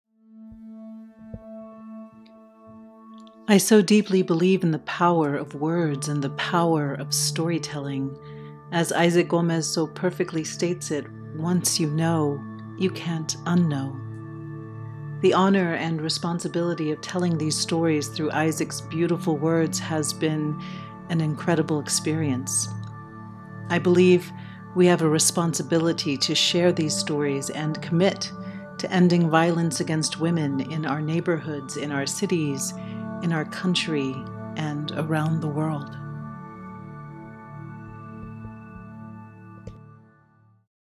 Thoughts on THE WAY SHE SPOKE with Michelle Lopez-Rios
Don't miss THE WAY SHE SPOKE by Isaac Gómez

An actor walks on stage, opens a script, and reads—and reality and performance blur as she shares harrowing details of the fates of las desaparecidas, the vanished women of Ciudad Juárez, Mexico. Haunting and visceral, yet profoundly compassionate, this challenging new play digs for the roots of an epidemic of violence that has resulted in the murders of thousands of women, and gives voice to a community rendered silent by fear.